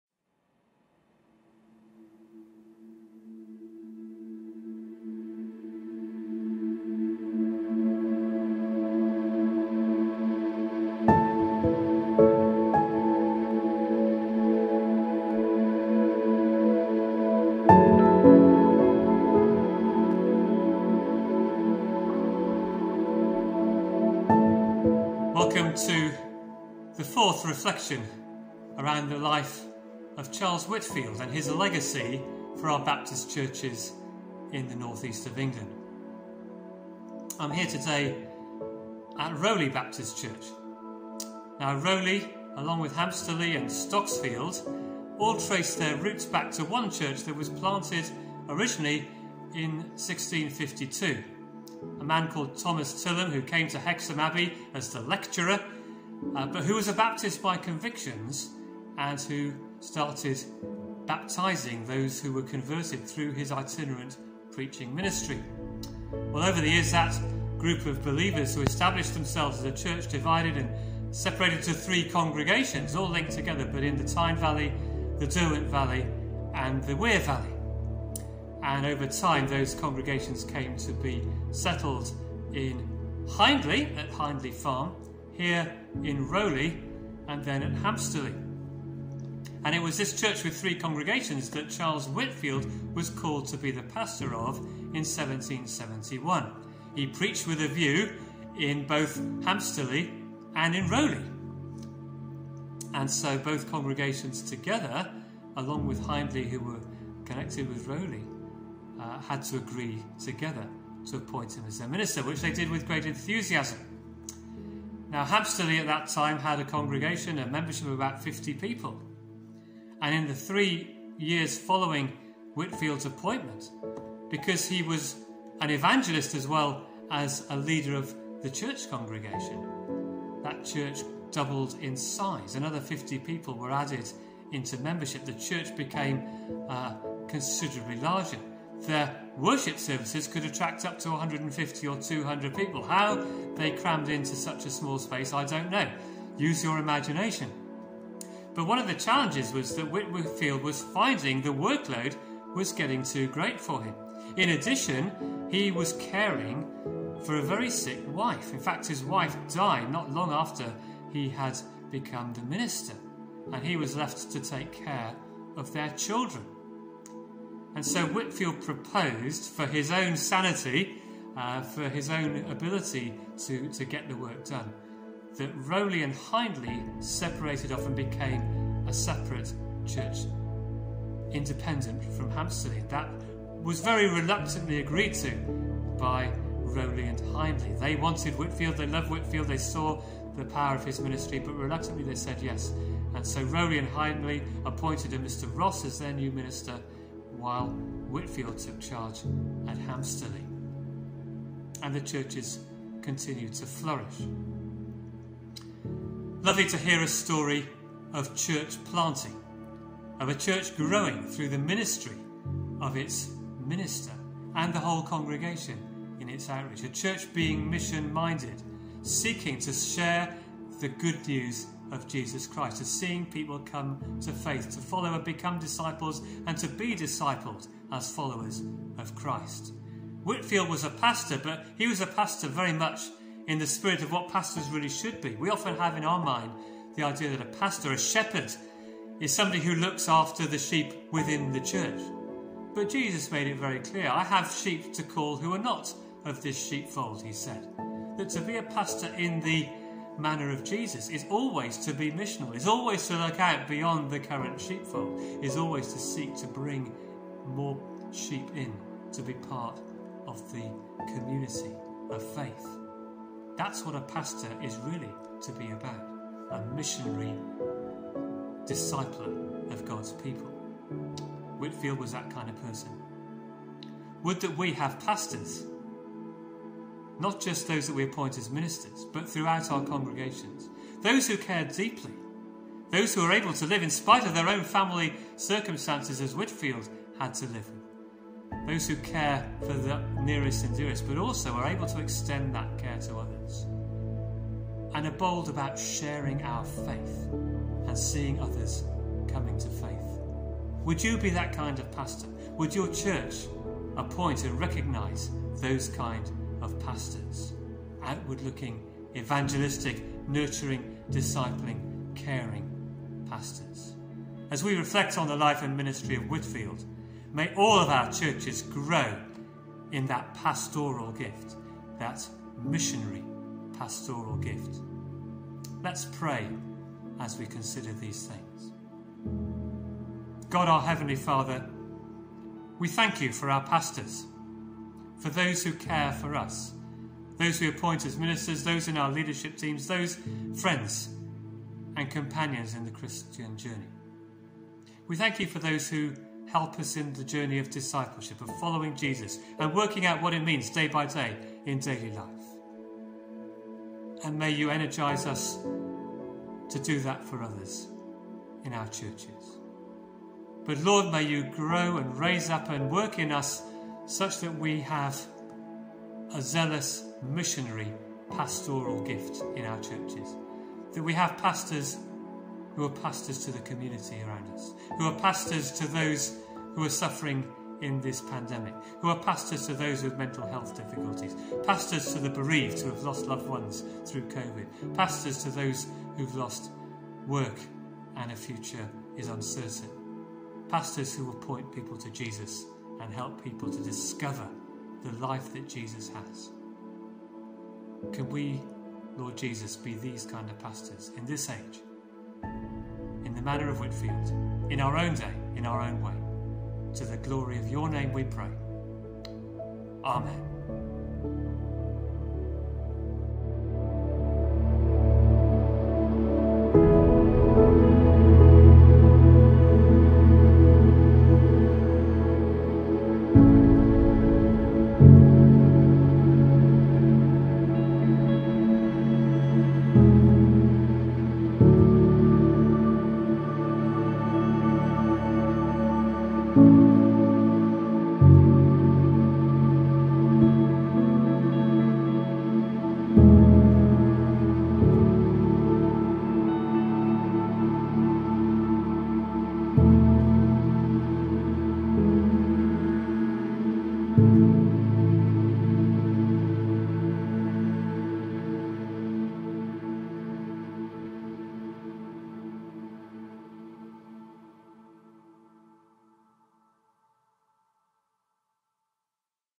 Whitfield Reflections 4
Further reflections on the life and ministry of Rev Charles Whitfield, this time focussing upon the need for pastors to have a missionary dimension to their work.